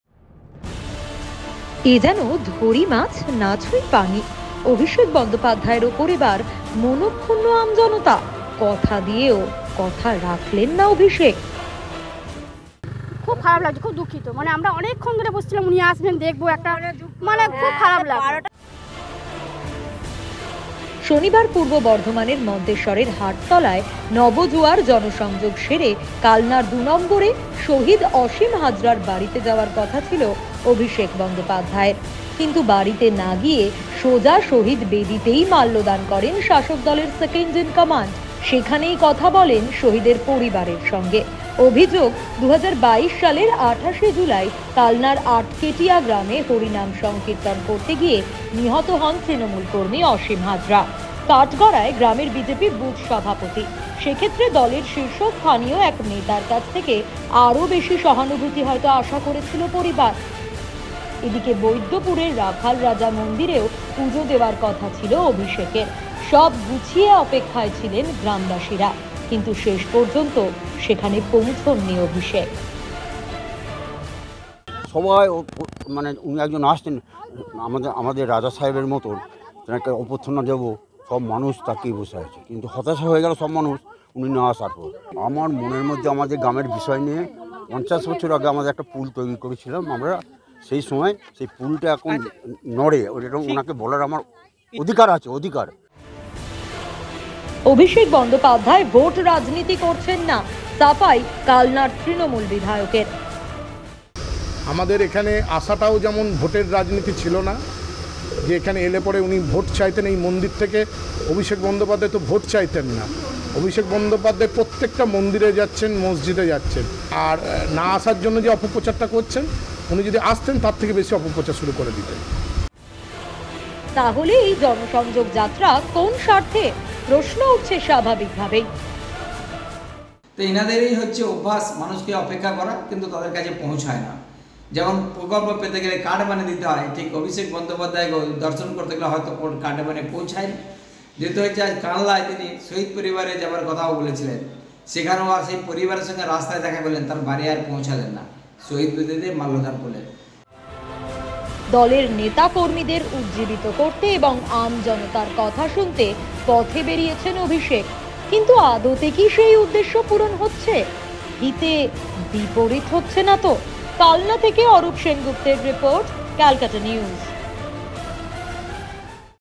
কথা রাখলেন না...অভিষেকের ওপর ক্ষুণ্ণ জনতা? | CN
কথা দিয়েও কথা রাখলেন না অভিষেক বন্দ্যোপাধ্যায়? ভাঙল আমজনতার ভরসা? পূর্ব বর্ধমানের মন্তেশ্বরের সভার পরই বিতর্কে অভিষেক।